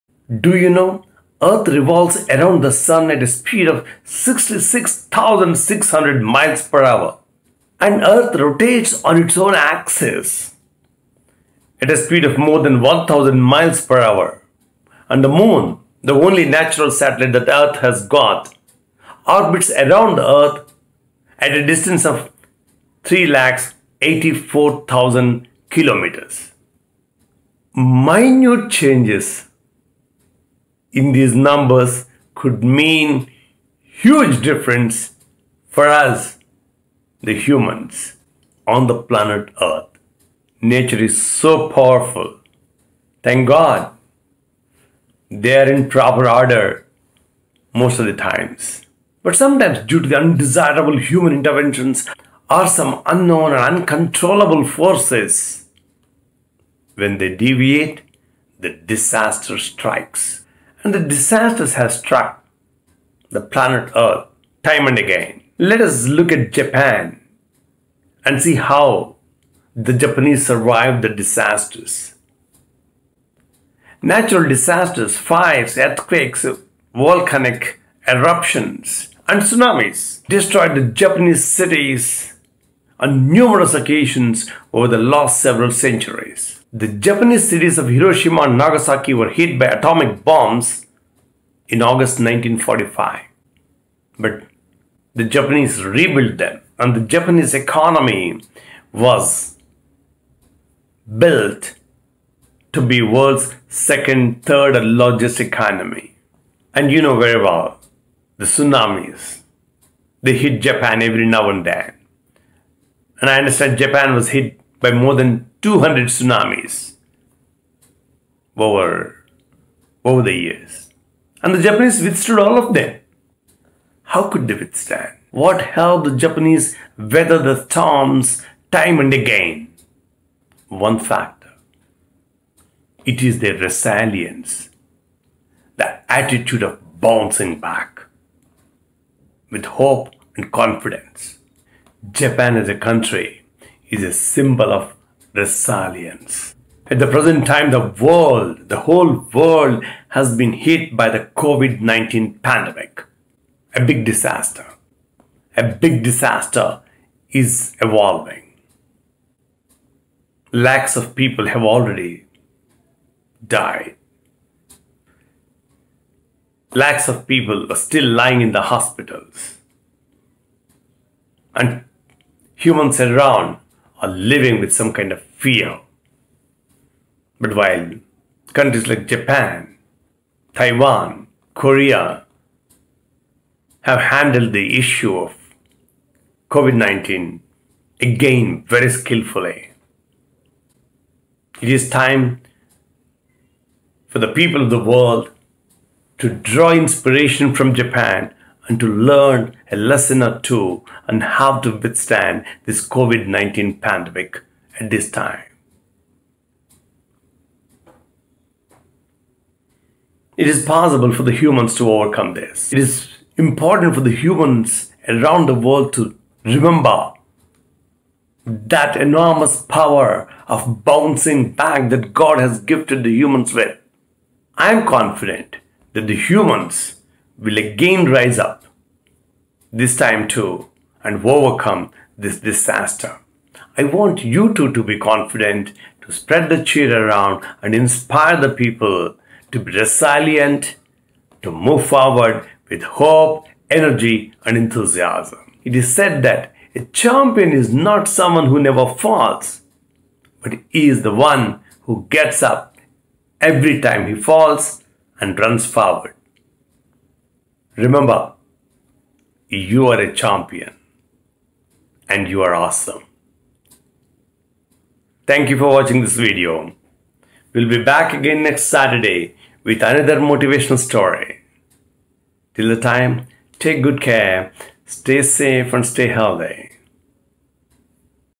Champions have this quality. And, you too have it. Are you aware of it? (Saturday Story From Siva)
Japanese have shown the world how to be resilient and bounce back disaster of disaster. It is time for all of us to realize that we too possess that strength of resilience and to withstand and overcome the COVID-19 disaster that the world is grappling with.